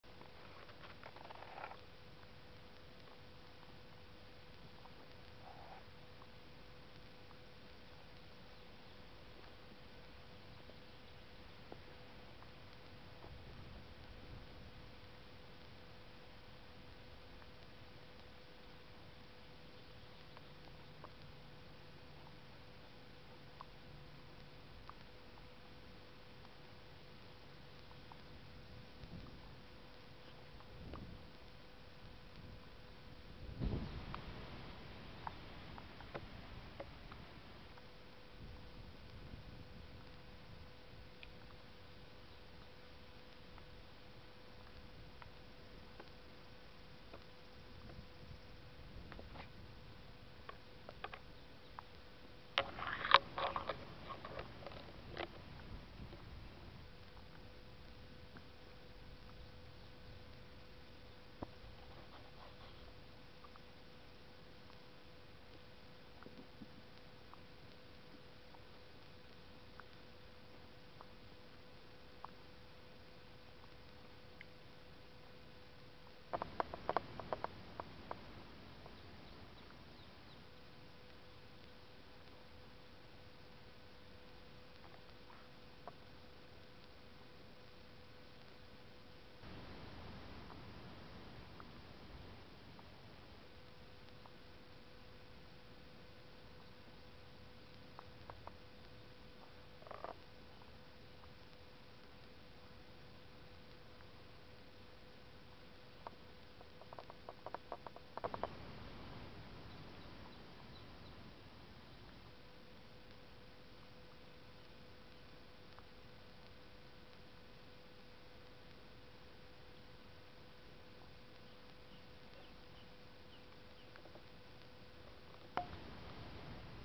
Leopard Tortoise and Box Turtle Share Strawberry
This is a video of my Box turtle colony comprised of 20-25 turtles. I went down to their pen for feeding time and fed them some strawberries. I was surprised to find my Leopard tortoise,Whily,and one of my Box turtles sharing a strawberry